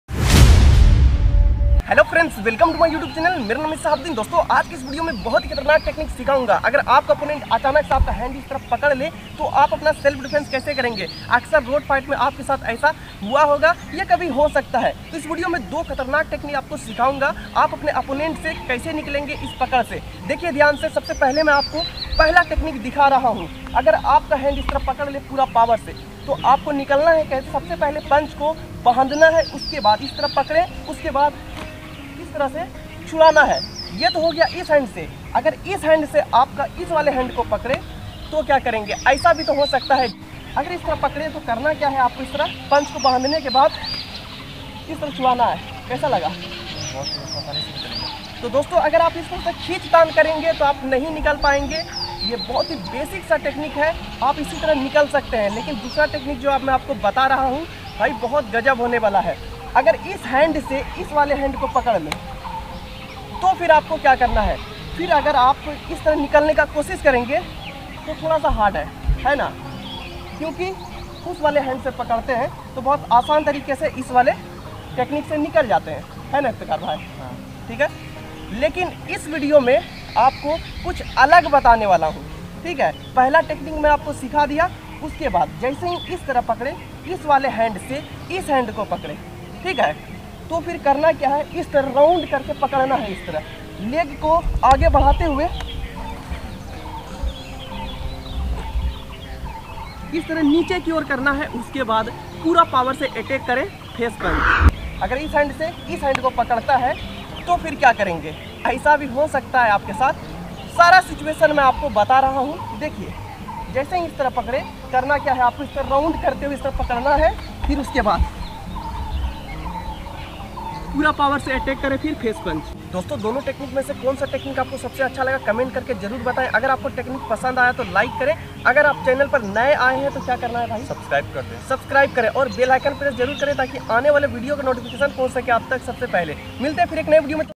How to defend hand || Self defence techniques || Online karate training || Shahabuddin karate
Dosto es video me Self defence techniques btaya gya hai hand Self defence kaise kare